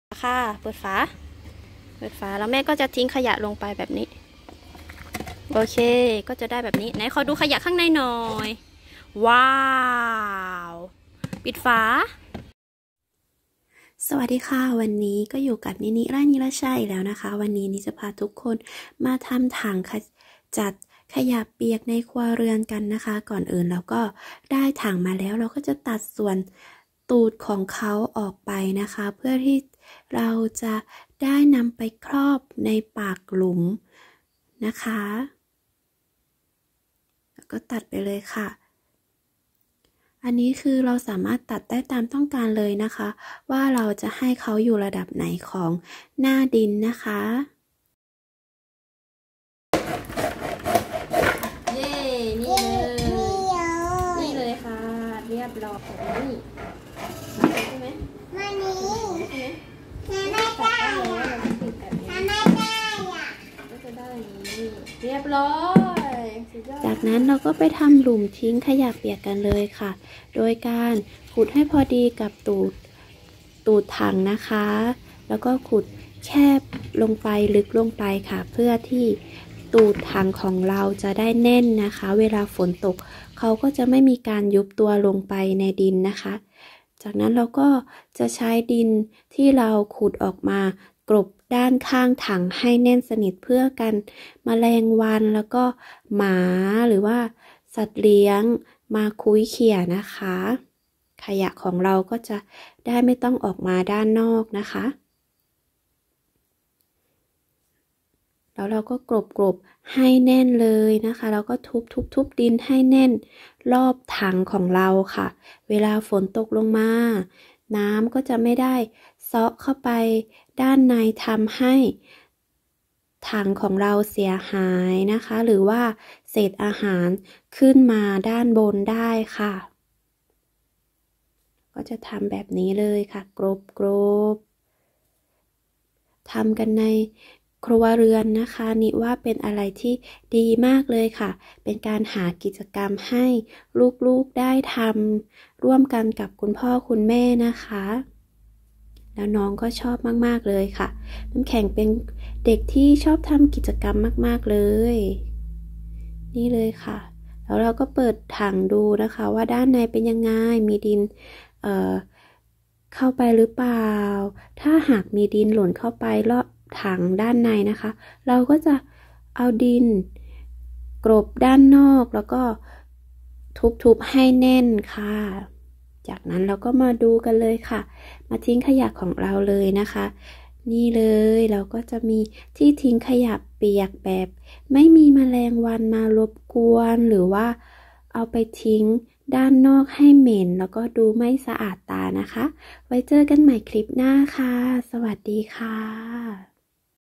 วิธีทำถังทิ้งขยะเปียกในครัวเรือน ลดโลกร้อน
วิธีทำถังทิ้งขยะเปียกในครัวเรือนง่ายๆ
ขอบคุณสำหรับการติดตามรับชมนะคะ ติดตามพูดคุยเพิ่มเติมได้ที่
เพจ Facebook
➡️ FBเพจ niniไร่ณิลชา